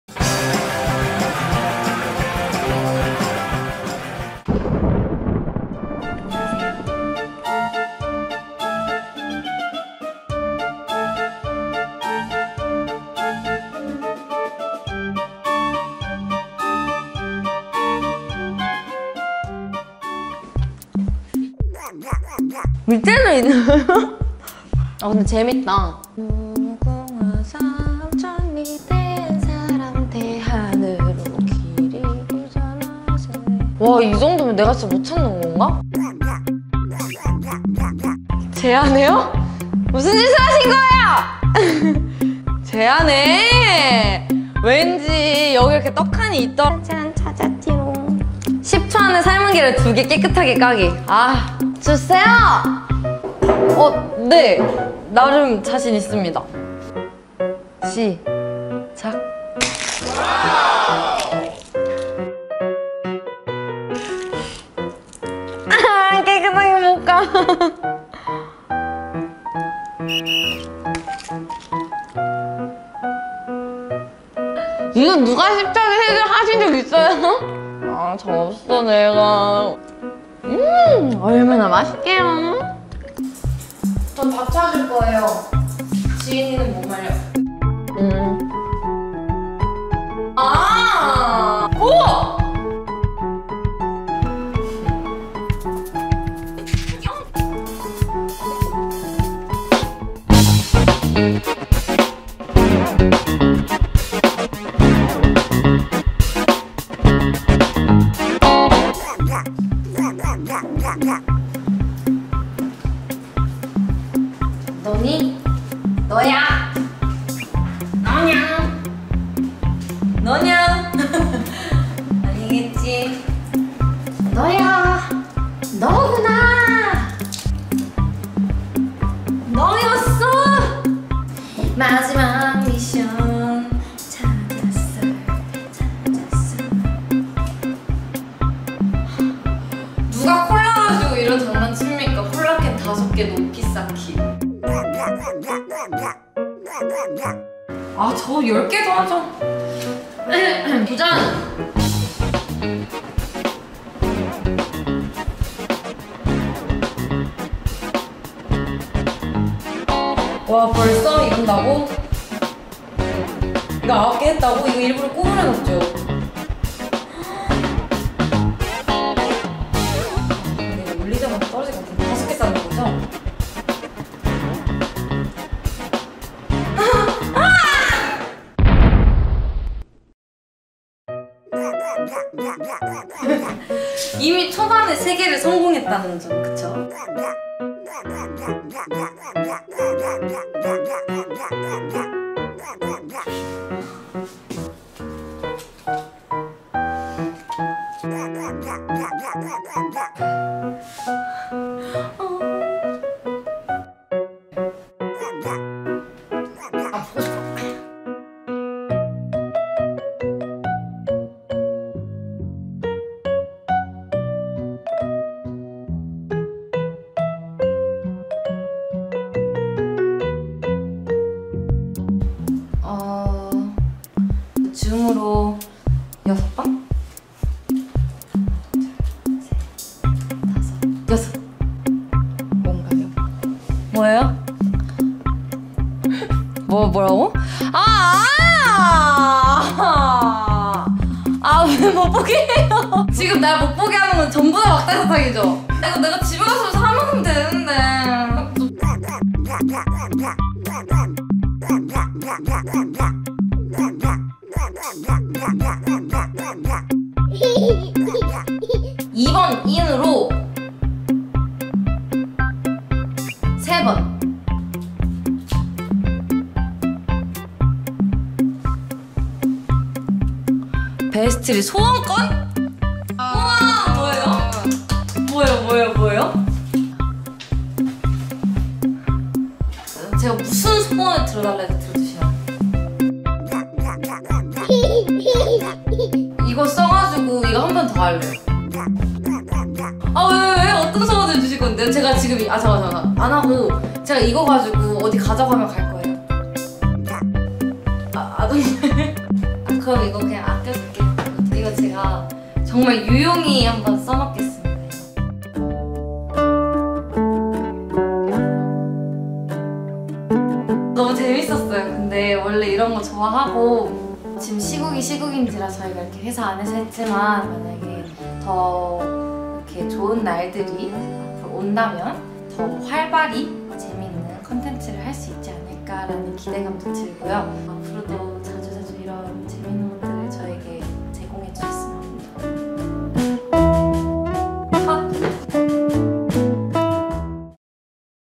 ★베스트TV★ 줌인지인(Zoom in JIIN) EP.2 보물찾기
★베스트TV★ 줌인지인(Zoom in JIIN) EP.1 보물찾기 - 두 번째 이야기 -

VAST 대표 요정 김지인✨을 더더더 가까이서 관찰하는
베스트TV의 뉴 프로젝트, 줌인지인 (Zoom in JIIN)!

숨겨진 보물 전부를 찾기 위해 사무실 탐방에 나선 지인🧐

얼마나 많은 미션에 성공할 지 궁금하신 분들께서는
지금 바로 플레이▶ 하세요!